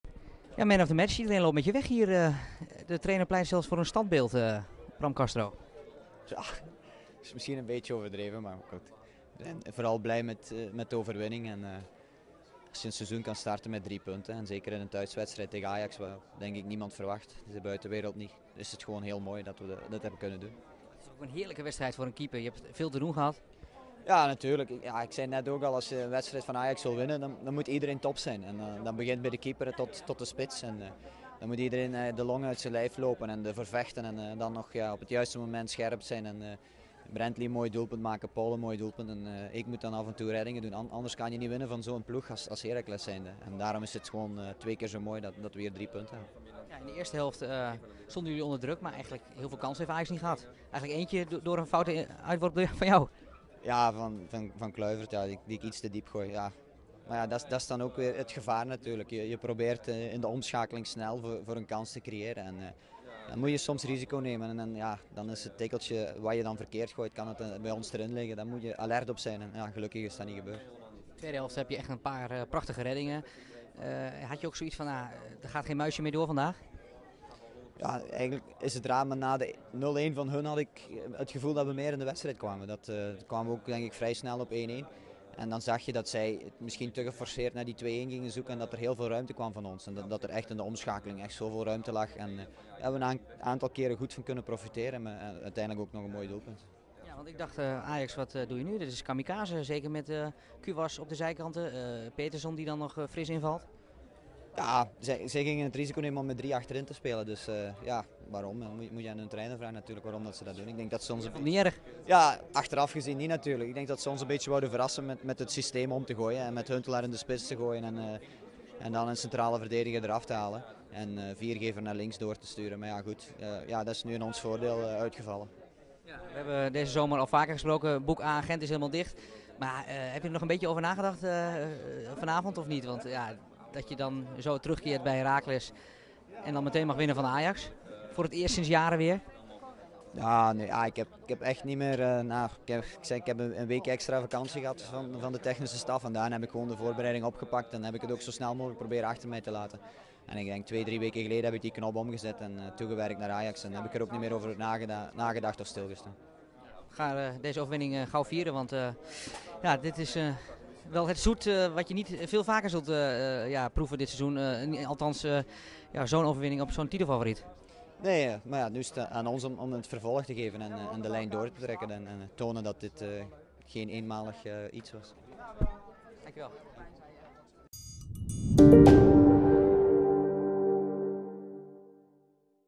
Bram Castro (Heracles) na heldenrol: "Tegen Ajax moet iedereen top zijn"
Bram Castro vertolkte in de eerste competitiewedstrijd een heldenrol door de 2-1 overwinning van Heracles Almelo op Ajax over de streep te trekken. De doelman was voor de Amsterdammers een grote sta-in-de-weg.